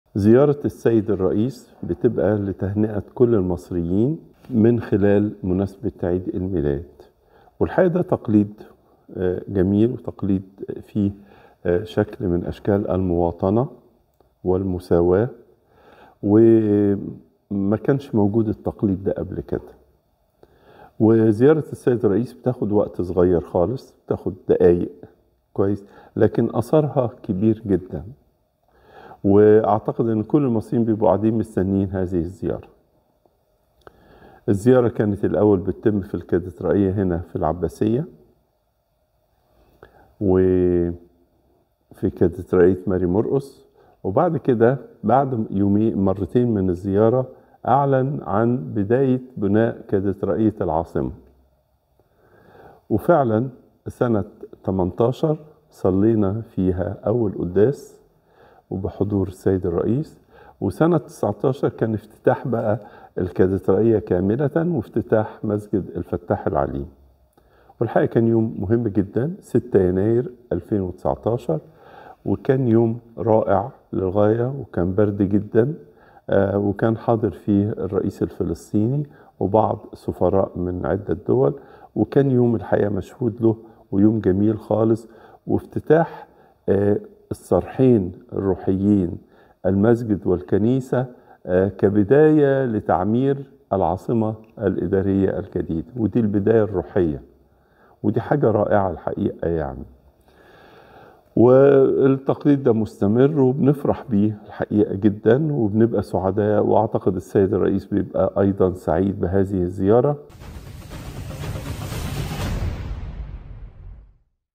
تقليد من أجل المواطنة والمساواة
البابا تواضروس يسرد تفاصيل زيارات الرئيس السيسي للتهنئة بعيد الميلاد

تابعوا حساباتنا على منصات التواصل الاجتماعي المختلفة